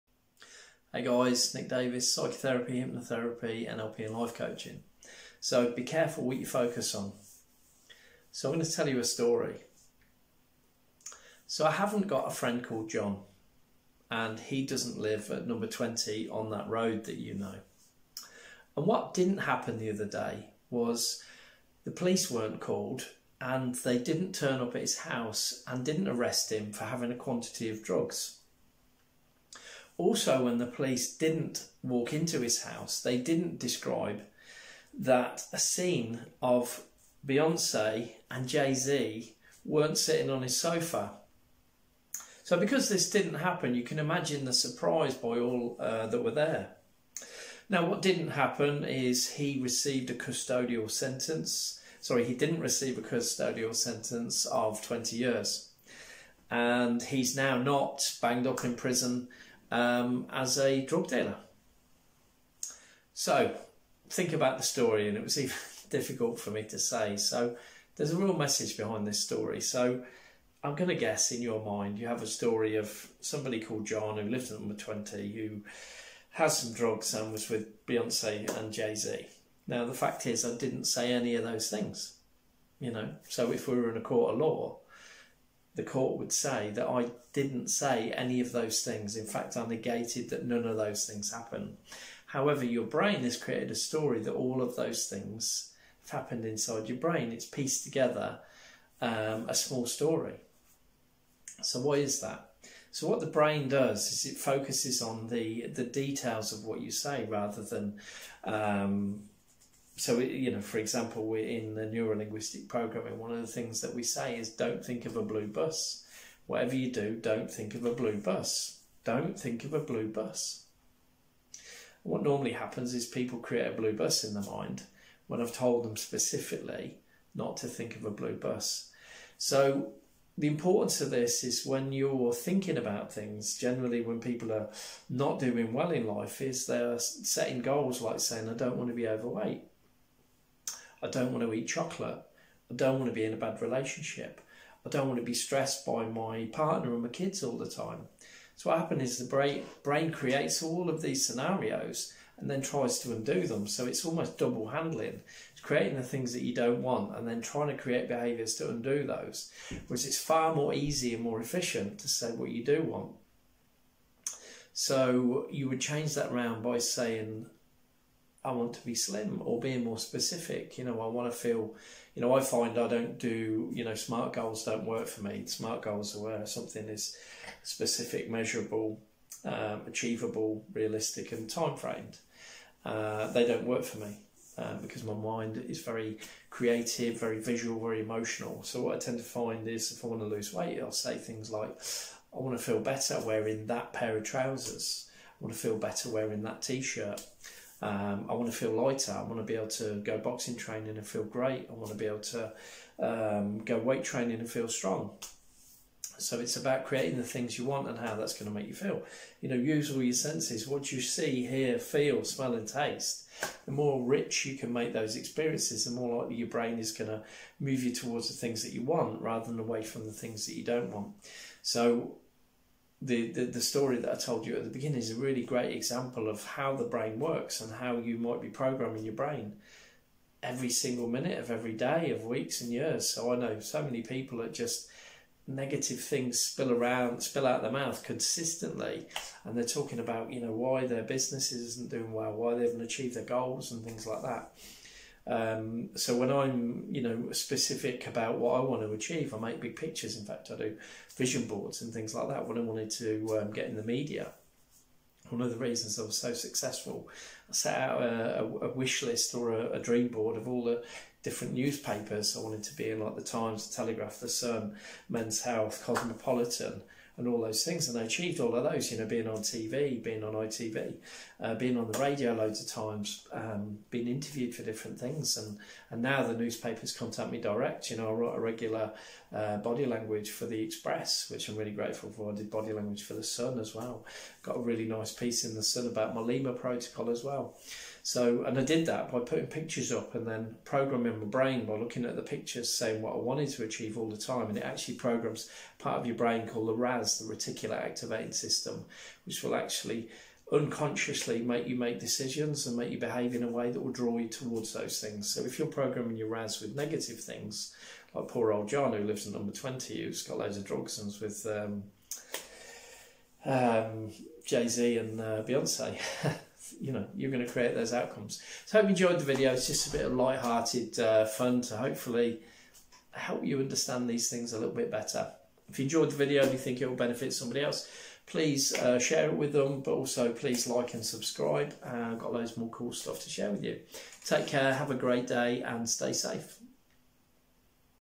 "Be Careful What You Focus On"
A short video explaining how focusing on avoiding negative outcomes can actually draw you to them.